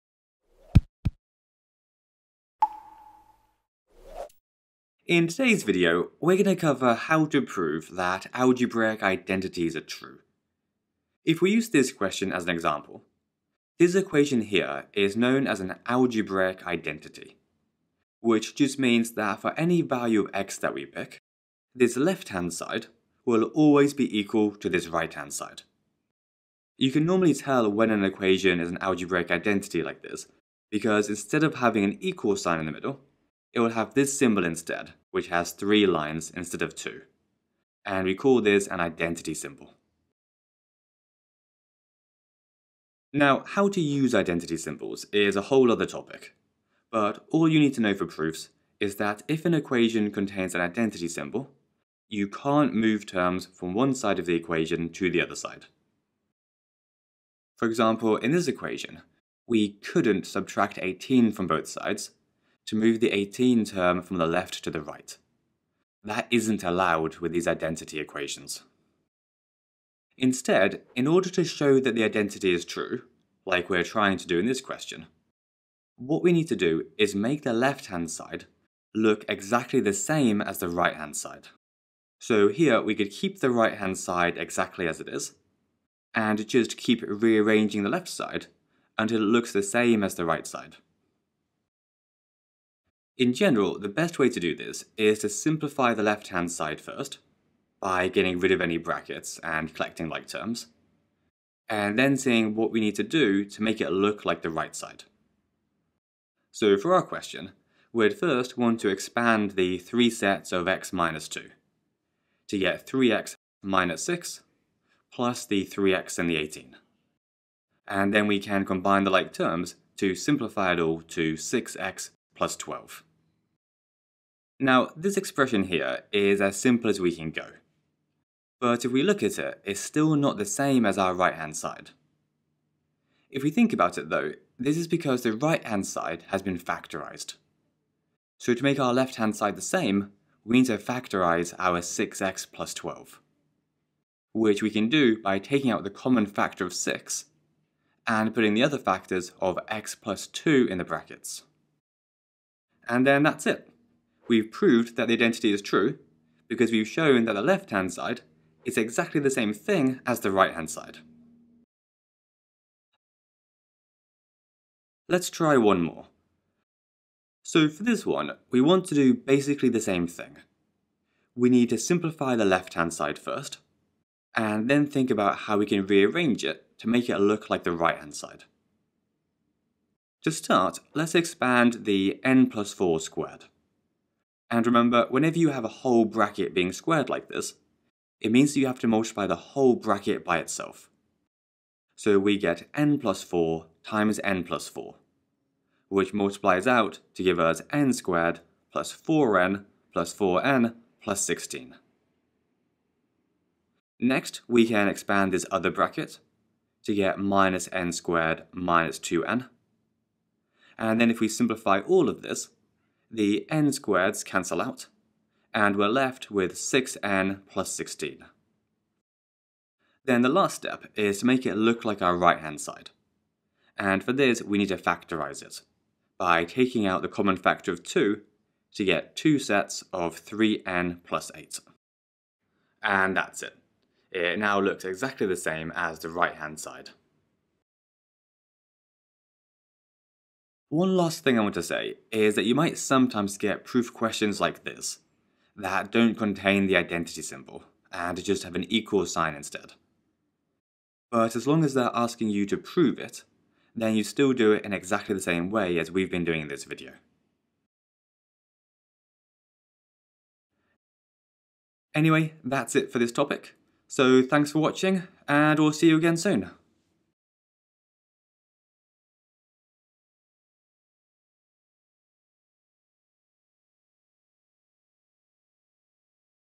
GCSE Maths - How to Prove Algebraic Identities - Proof Part 2 (2026/27 exams)
*** WHAT'S COVERED ***
1. Understanding Algebraic Identities.
2. Rules for Working with Identities.
3. Method for Proving Algebraic Identities.
    * Manipulating one side algebraically until it is identical to the other side.
    * Using techniques like expanding brackets, simplifying by collecting like terms, and factorising.
4. Worked Examples.
5. Notation in Proof Questions.

KS3 - Not on your course
GCSE Foundation - Not on your course

*** CHAPTERS ***
0:00 Intro: What is an Algebraic Identity?
0:32 The Identity Symbol
0:43 Key Rule: Don't Move Terms Across the Identity Symbol
1:10 How to Prove an Identity: Make One Side Match the Other
1:33 General Strategy: Simplify Left Hand Side
1:46 Example 1
2:44 Example 2
4:00 Note on Proof Questions Using '=' Instead of '≡'